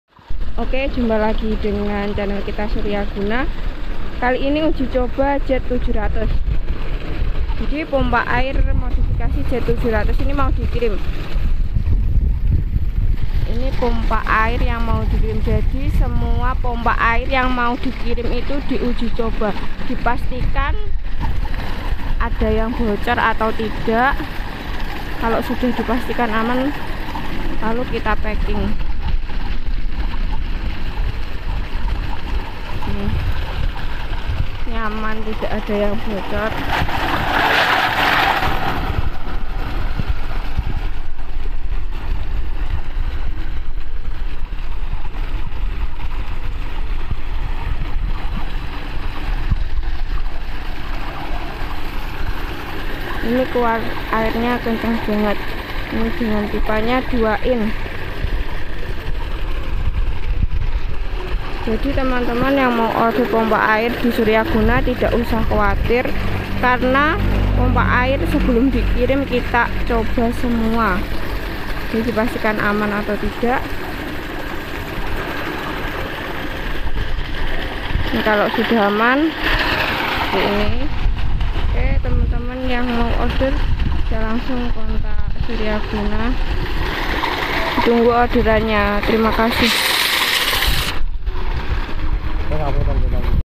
Tes Pompa Sebelum Dikirim
Pompa Air Modifikasi Jet700 II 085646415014

Pompa air modifikasi dari surya guna sebelum dikirim selalu dicoaba terlebih dahulu . dipastikan tidak ada yang bocor dan aman. jadi ntuk teman2 yang mau order pompa modifikasi di surya guna tidak usah bingung, karna sebelum dikirim selalu kami coba terlebih dahulu. silahkan diorder...